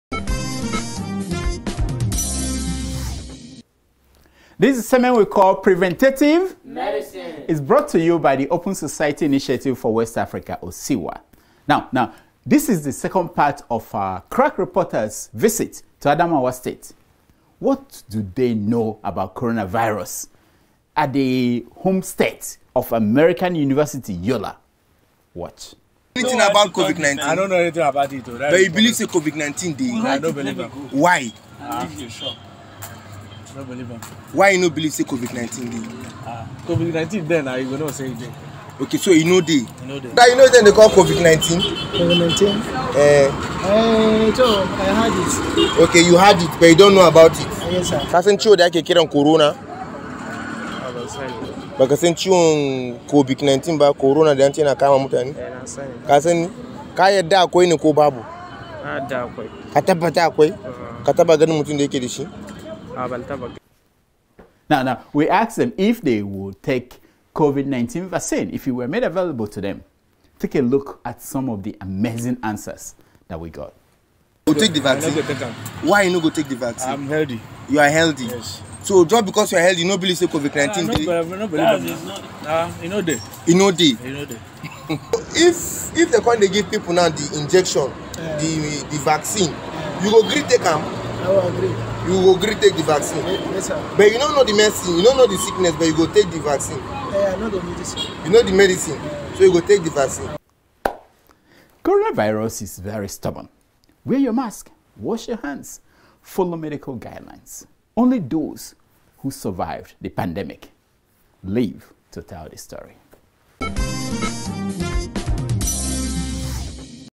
Preventative Medicine 31: Part two of our crackreporter's report from Adamawa State
In continuation of our coverage of the COVID-19 pandemic in Nigeria, we sent our crackreporter to Adamawa to investigate how they are dealing with the pandemic. The reaction was quite interesting. As always, Dr. Damages reminds Nigerians to remain vigilant.